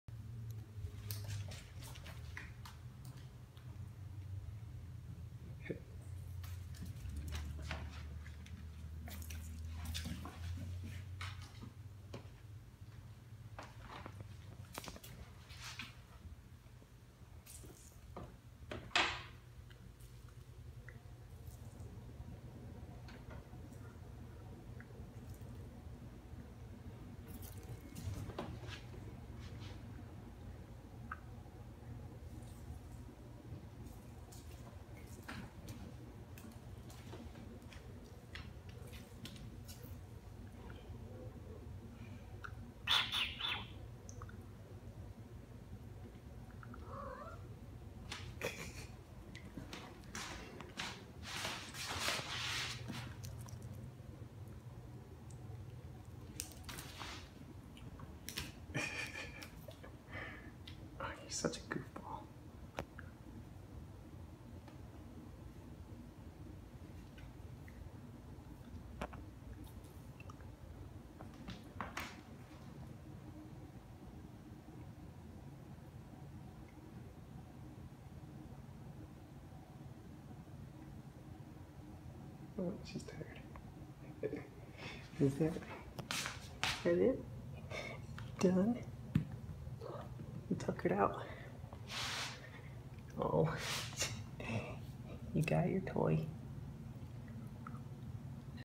Blind cat hockey skills
Rey plays with a gum wrapper and never ceases to amaze me with her spatial awareness.